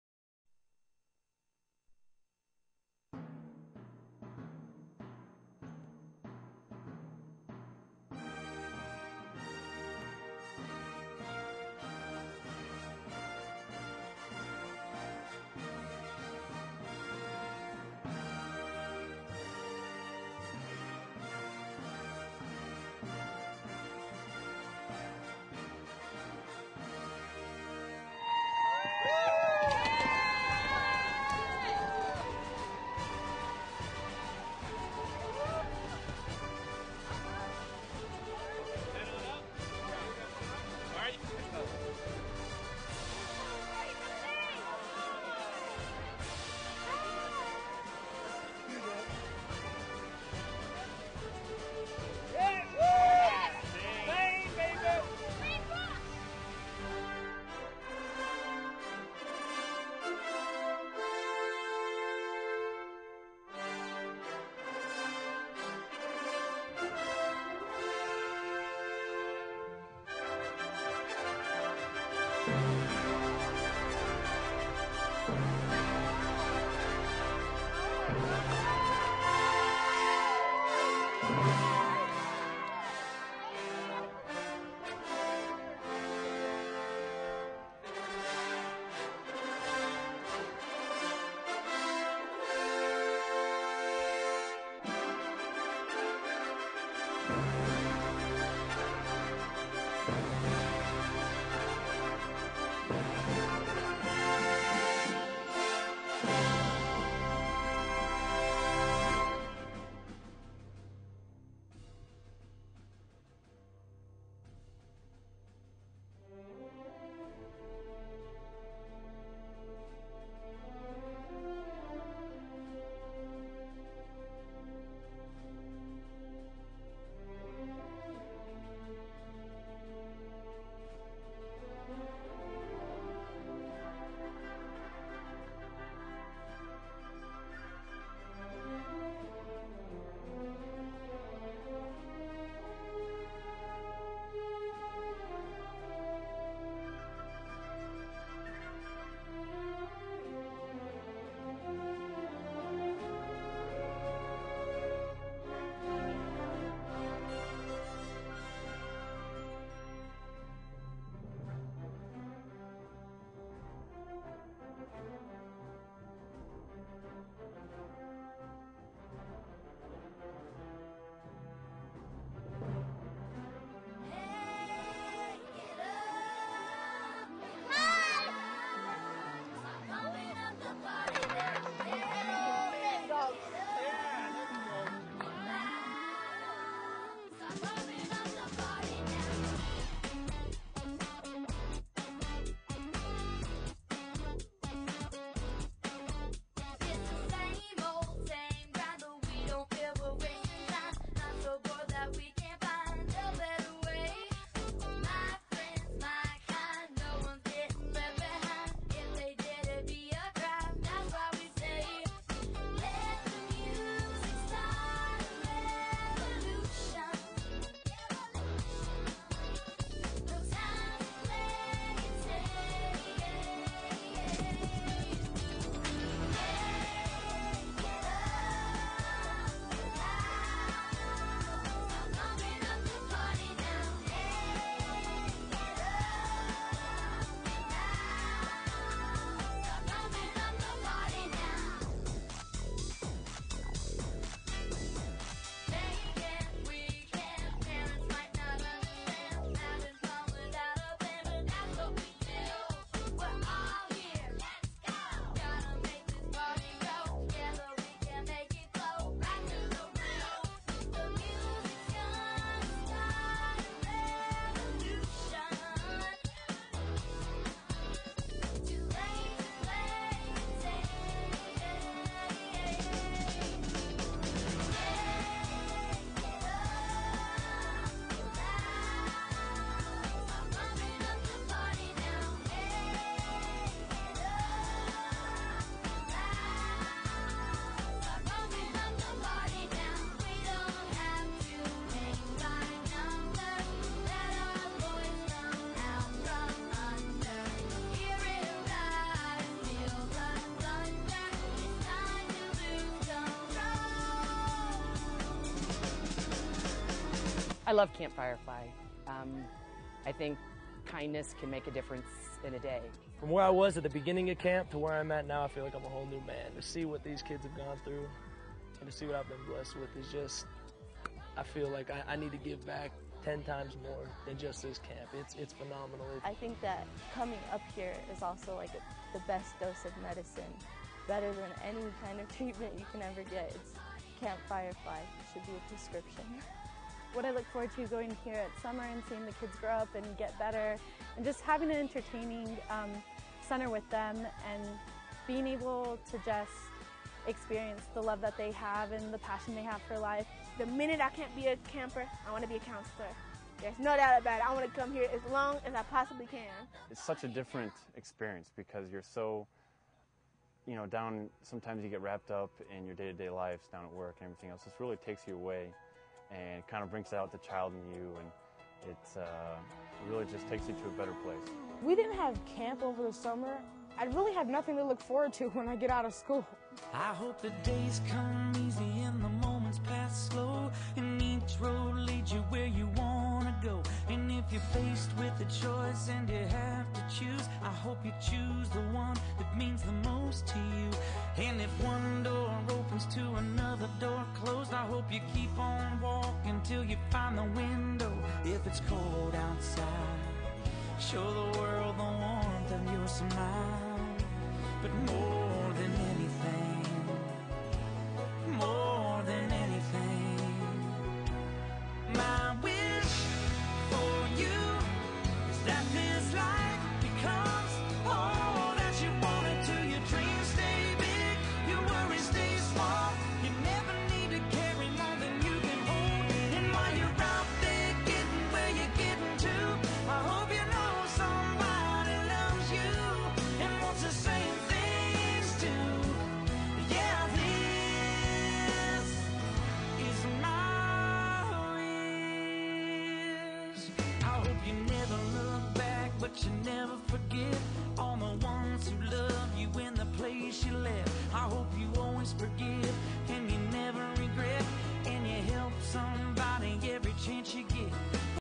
Camp Firefly Part 1
Colliers International Las Vegas brokerage helps out at Camp Firefly through our on-going Links for Life Foundation. You can learn more about Camp Firefly by visiting their You can learn more about Collier's Links for Life Foundation by visiting